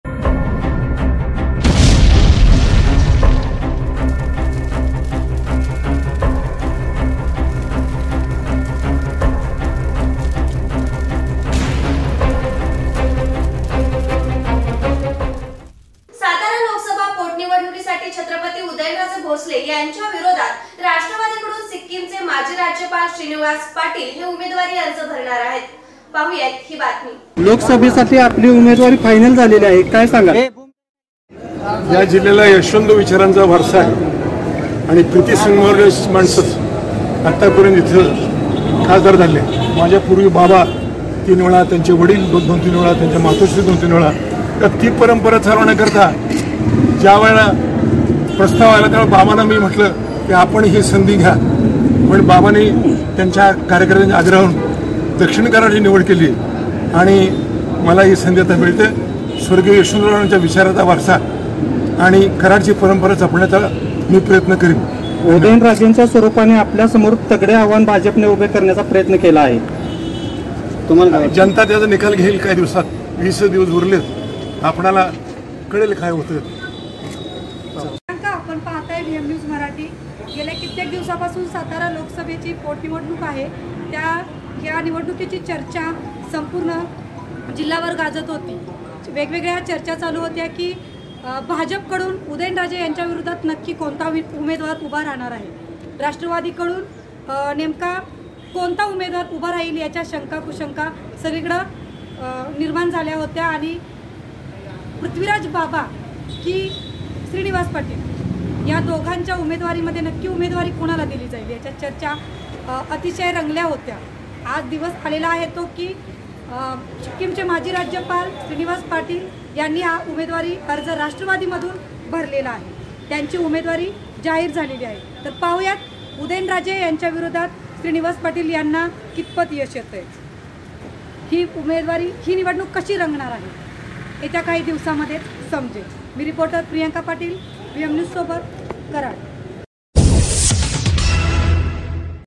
कोणता उमेदवार तगडा आहे आणि जनता कोणाच्या पाठीमागे आहे हे जनता दाखवून देईल-मा.रा. श्रीनिवास पाटील
कोणता उमेदवार तगडा आहे आणि जनता कोणाच्या पाठीमागे आहे हे जनता दाखवून देईल-मा.राज्यपाल श्रीनिवास पाटील .VM News Marathi